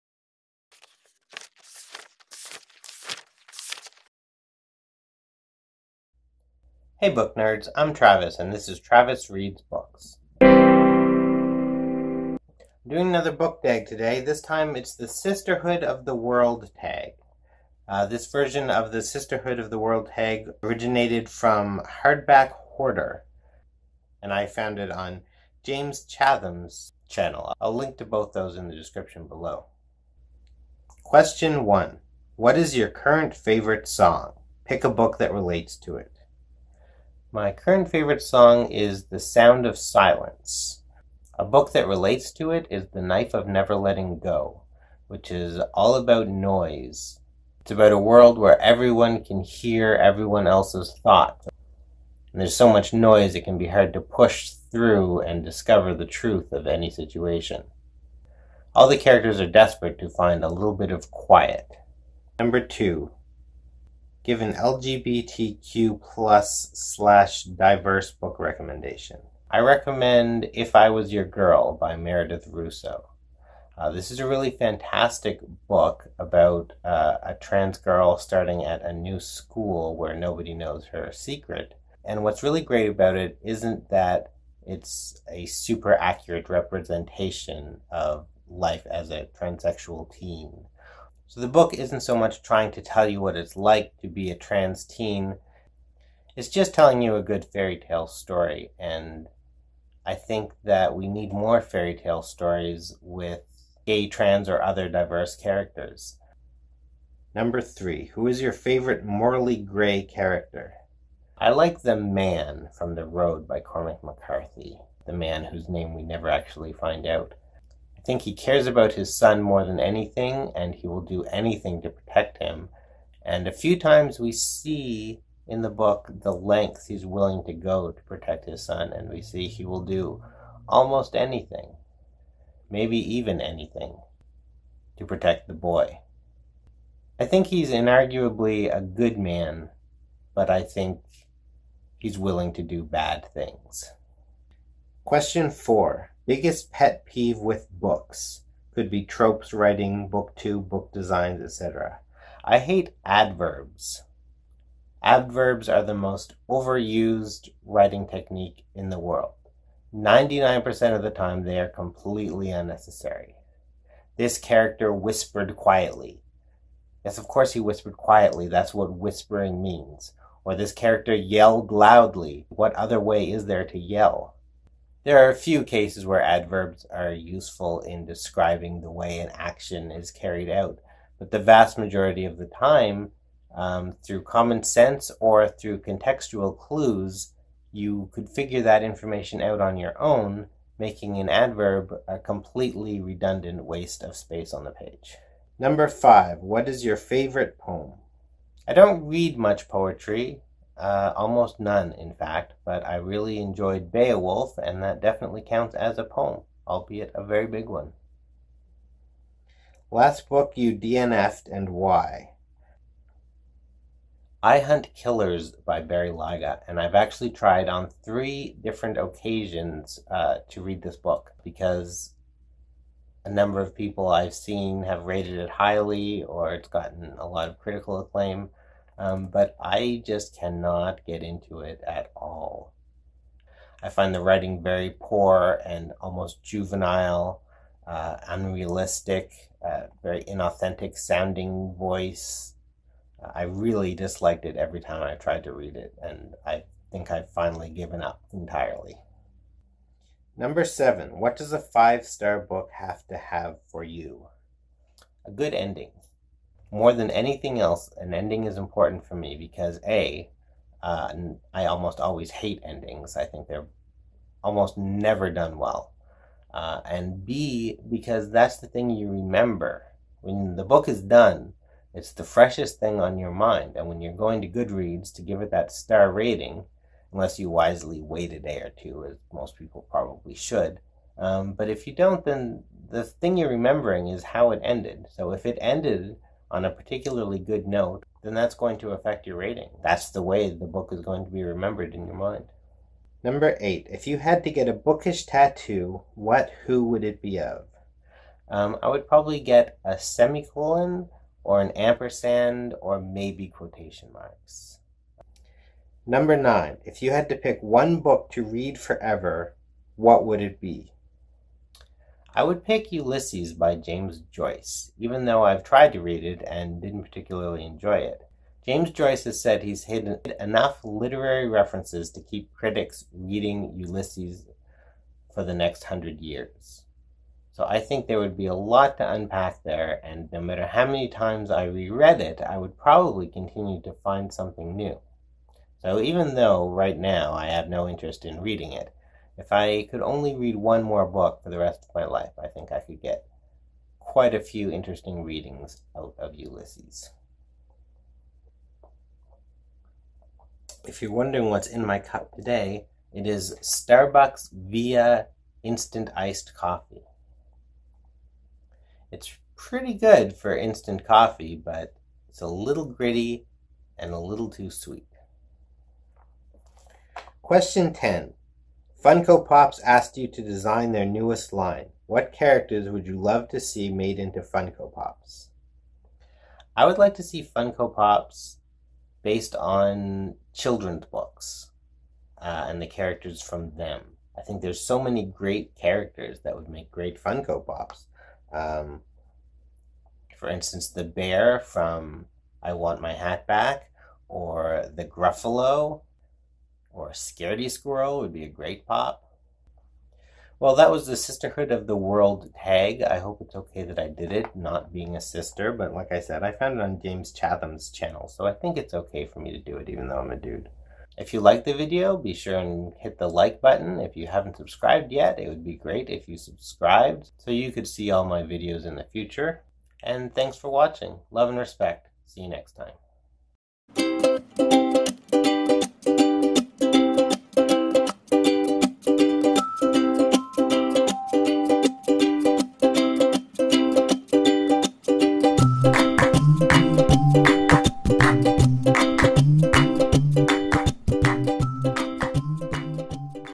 The "Sisterhood of the World" Tag (Am I allowed to do this one?)
Though not technically a sister, I do the Sisterhood of the World tag anyway. This version of the tag was originally made by
HardbackHoarder and I found out about it through James Chatham. Hope you enjoy!
ME:
OwlCrate(If you order an OwlCrate by clicking on this link, I get a third of a free crate)